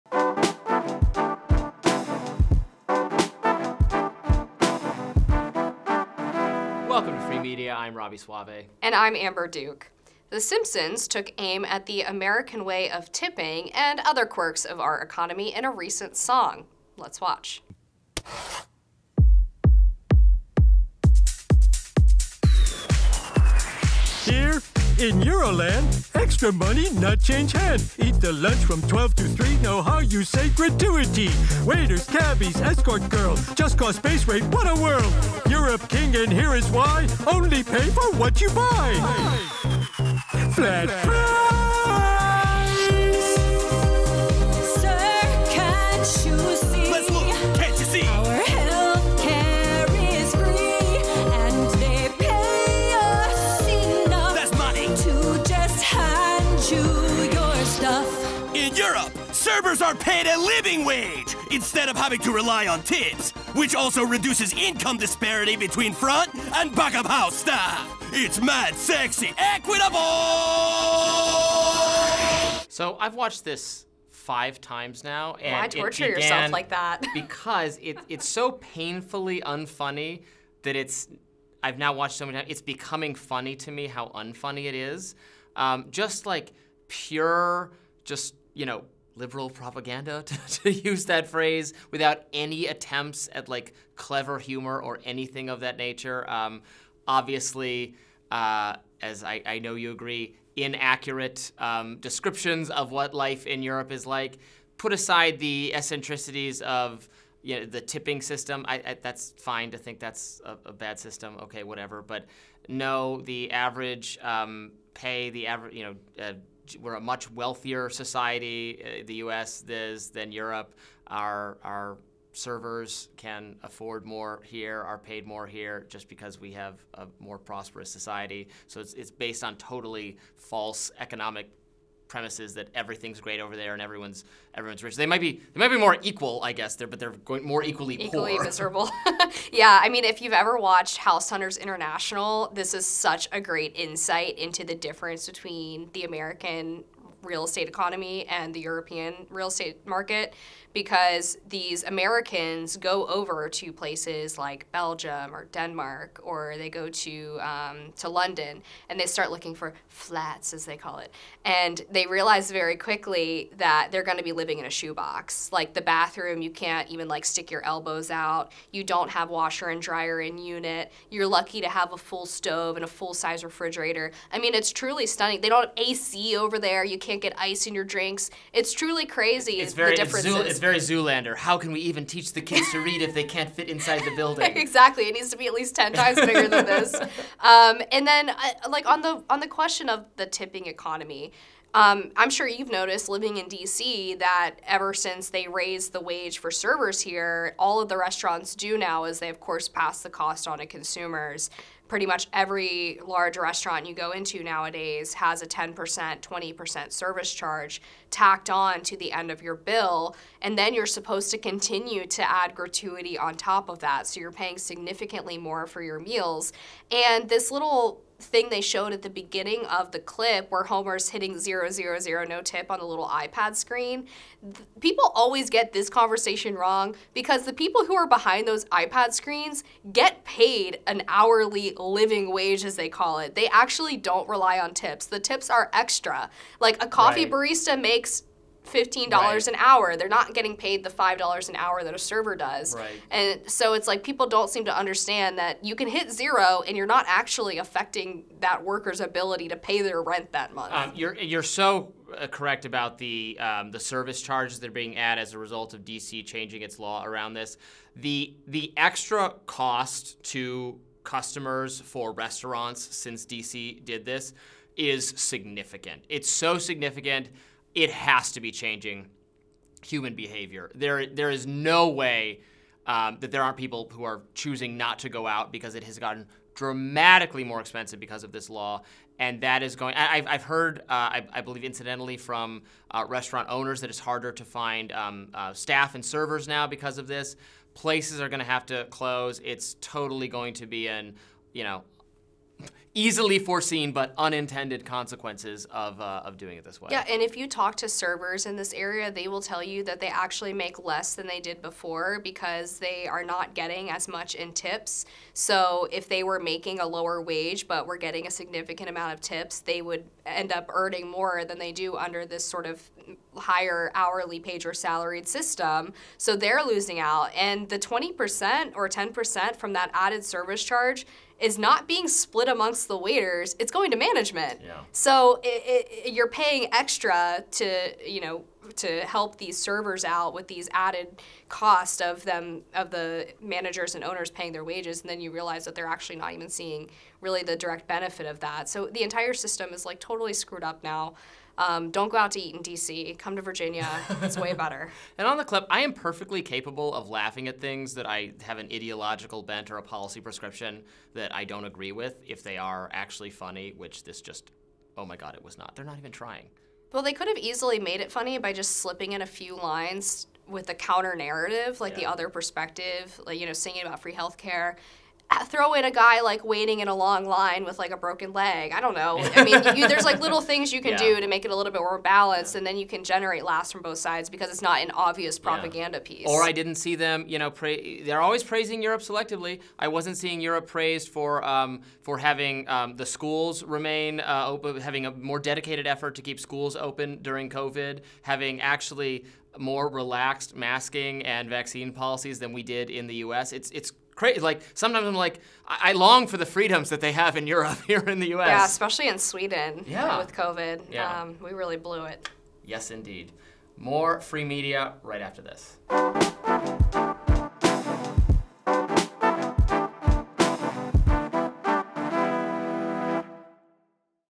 The Simpsons FACEPLANTS with misguided LIBERAL PROPAGANDA, Europe WORSHIP
Robby Soave and Amber Duke compare European and American economies.

Director of Photography: Alex Rosen
Producer: Veronica Riccobene
Editor: Chris Sowick

Illustration: Fox Network